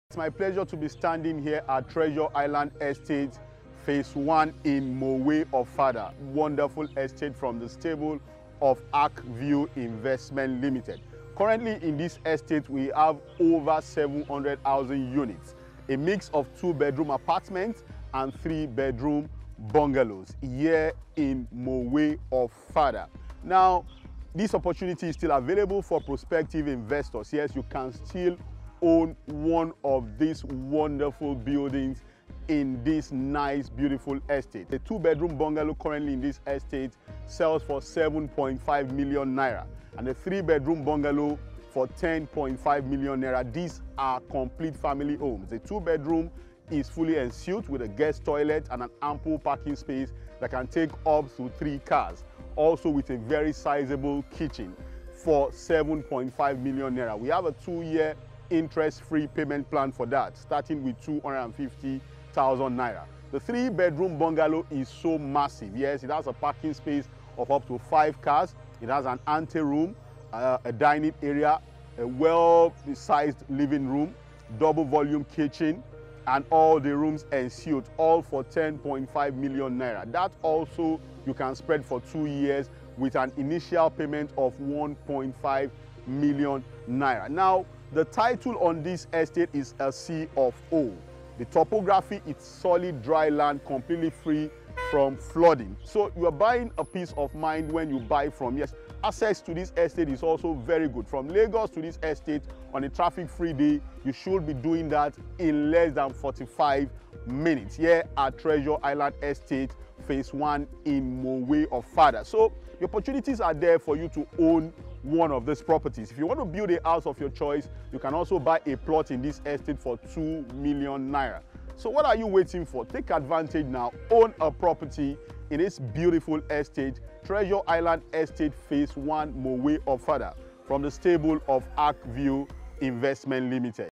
Affordable housing in Lagos, mowe-ofada
Own a 2beeroom and 3bedroom executive bungalow for 7.5M and 10.5M respectively with a 24months  no interest flexible payment plan

*Contact emiajeno homes & assets